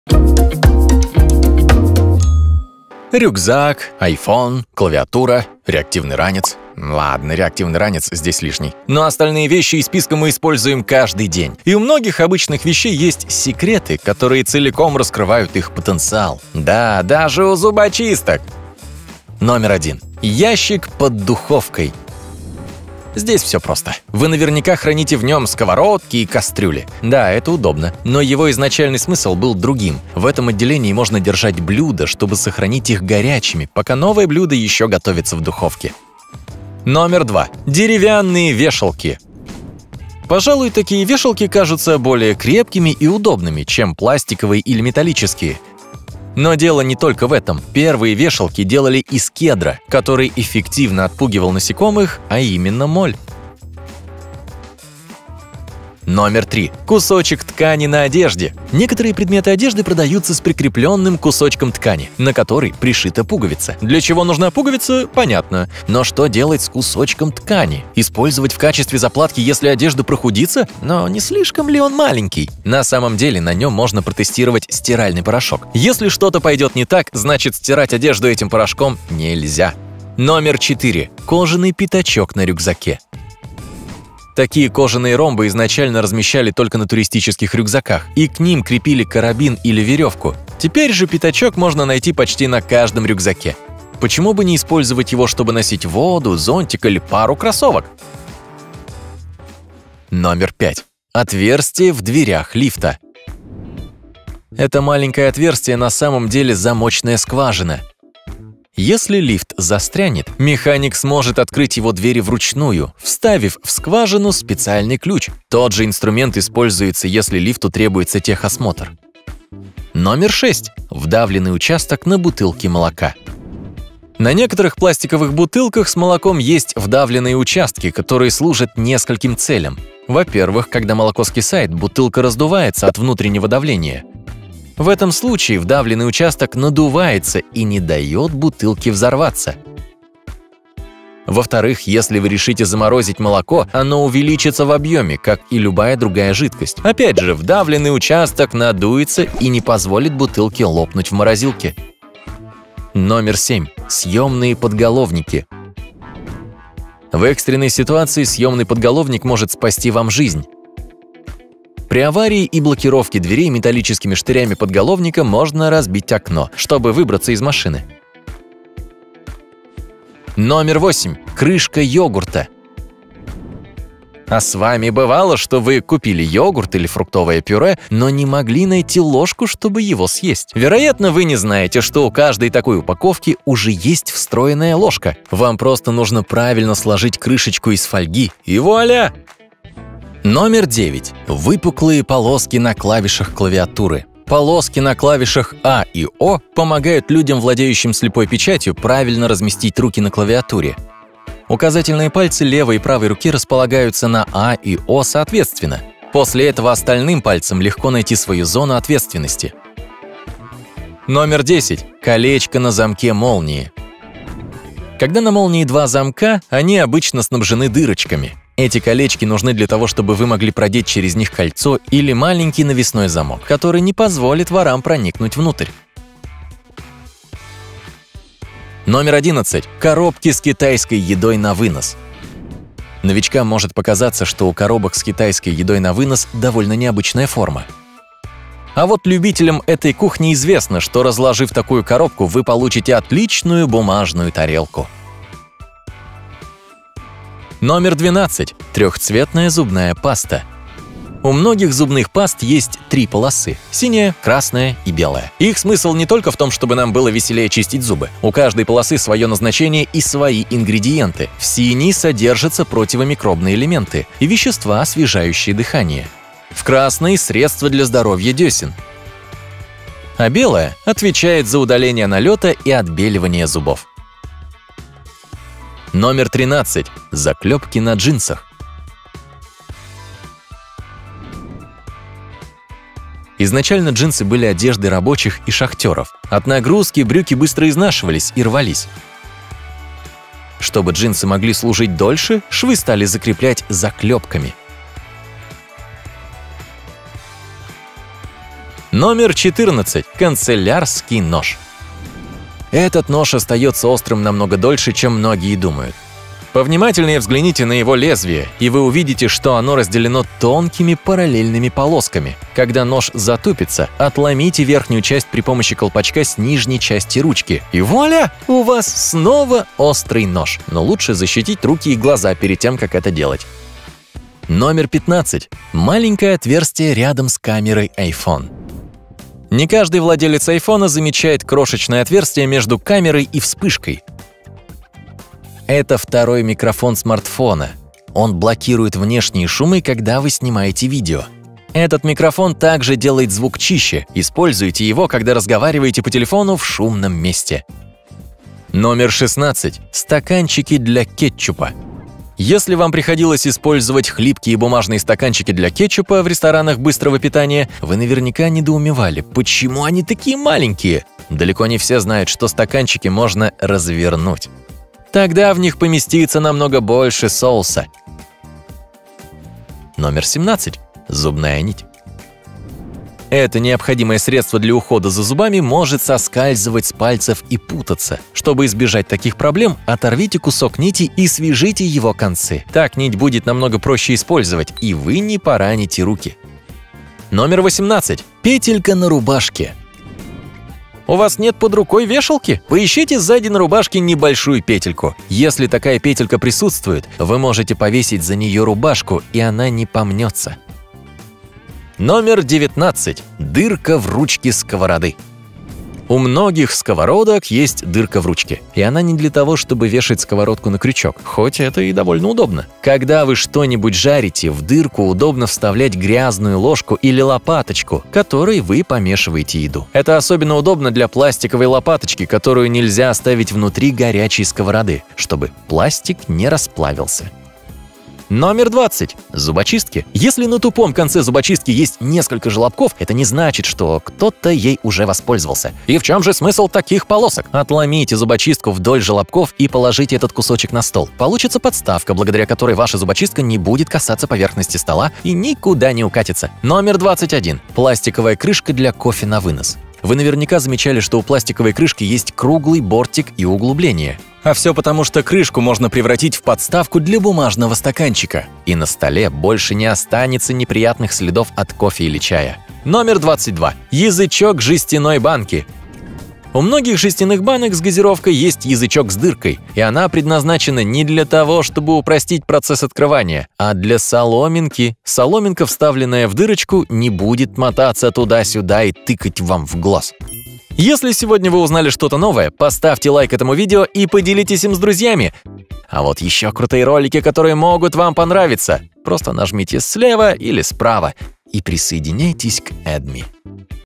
22 обычные вещи со скрытыми возможностями
Рюкзак, айфон, клавиатура, реактивный ранец. Ладно, реактивный ранец здесь лишний. Но остальные вещи из списка мы используем каждый день. И у многих обычных вещей есть секреты, которые целиком раскрывают их потенциал.

Если вам приходилось использовать хлипкие бумажные стаканчики для кетчупа в ресторанах быстрого питания, вы наверняка недоумевали, почему они такие маленькие. Далеко не все знают, что эти стаканчики можно развернуть — тогда в них поместится намного больше соуса. Интересно? Тогда давайте вместе узнаем о скрытых возможностях многих популярных вещей.

Тайм-коды:

Ящик под духовкой 0:19

Деревянные вешалки 0:37

Кусочек ткани на одежде 0:56

Кожаный «пятачок» на рюкзаке 1:22

Отверстие в дверях лифта 1:40

Вдавленный участок на бутылке молока 2:01

Съемные подголовники 2:34

Крышки йогурта 2:50

Выпуклые полоски на клавишах клавиатуры 3:12

Колечко на замке молнии 3:33

Коробки с китайской едой на вынос 3:50

Трехцветная зубная паста 4:09

Заклепки на джинсах 4:40

Канцелярский нож 5:02

Маленькое отверстие рядом с камерой на iPhone 5:25

Стаканчики для кетчупа 5:49

Зубная нить 6:09

Петелька на рубашках 6:27

Дырка в ручке сковороды 6:42

Зубочистки 7:09

Пластиковая крышка для кофе на вынос 7:26

Язычок жестяной банки 7:44

Анимация разработана AdMe.